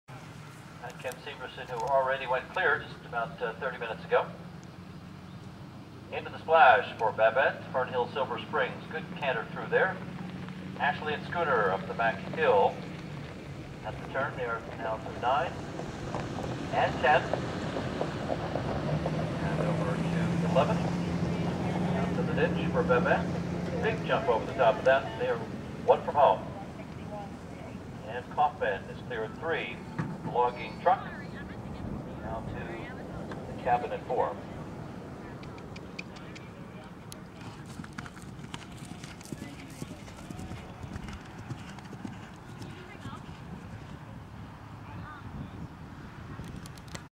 Babette Lenna VHT5.21.11.3.MOD
Babette & Dex at the water and finishing XC at VHT May 2011